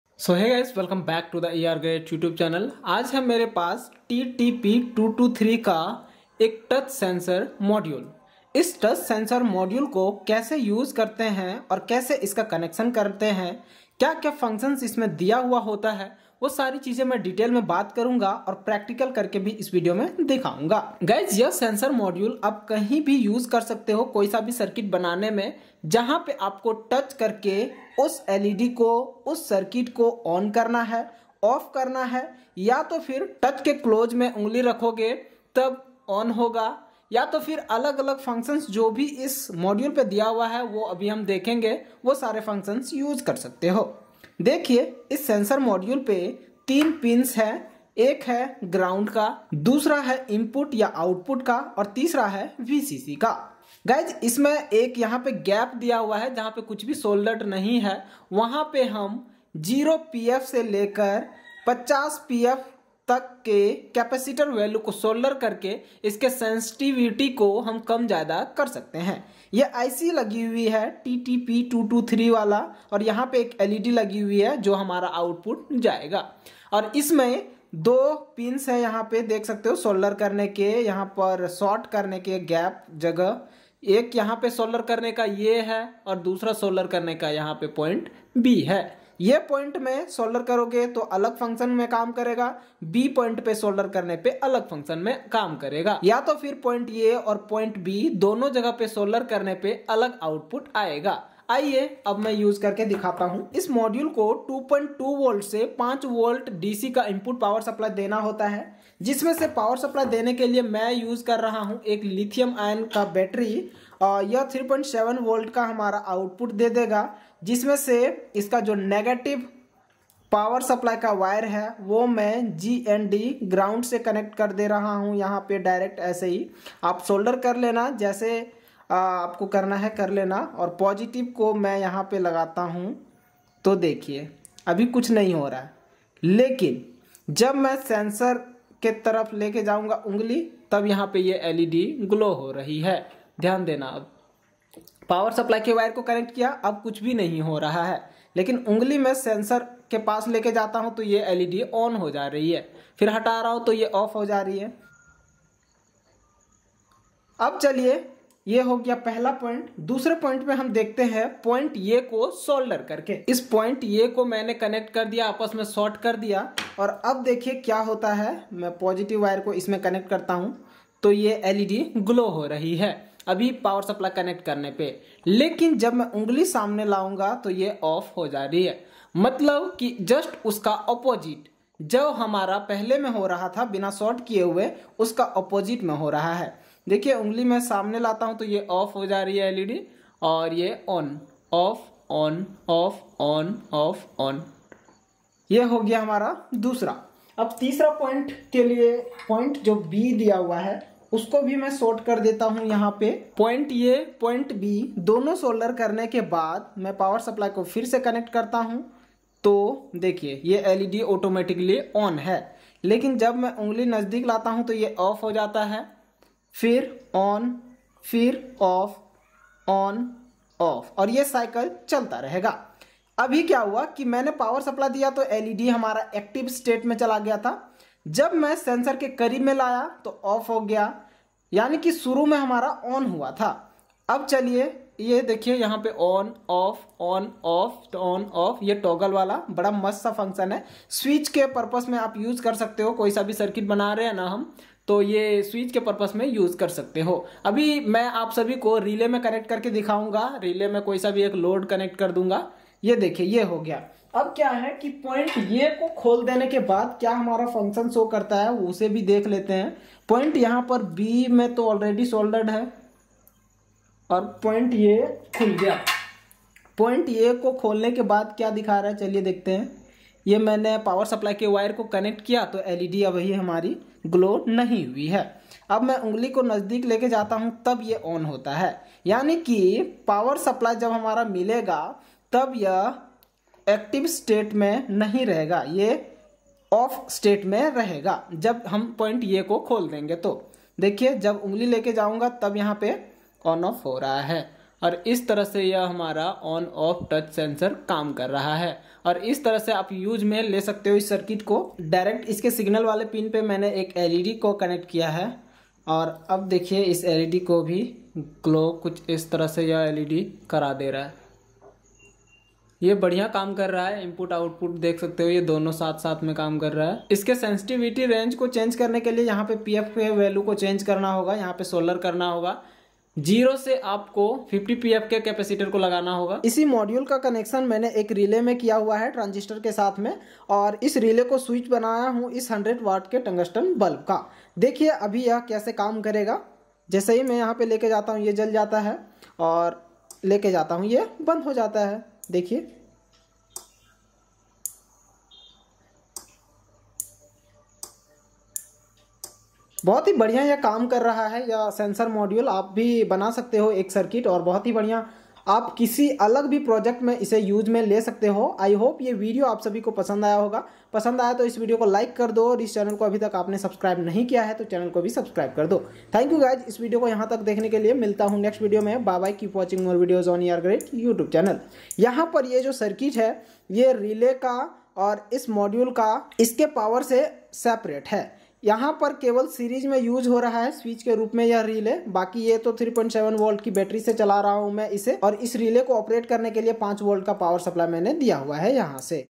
ये है TTP223 को Use करने का सही तरीका || How to use TTP223 Module Touch sensor in a Circuit in Hindi
ये है TTP223 को Use करने का सही तरीका || How to use TTP223 Module in a Circuit in Hindi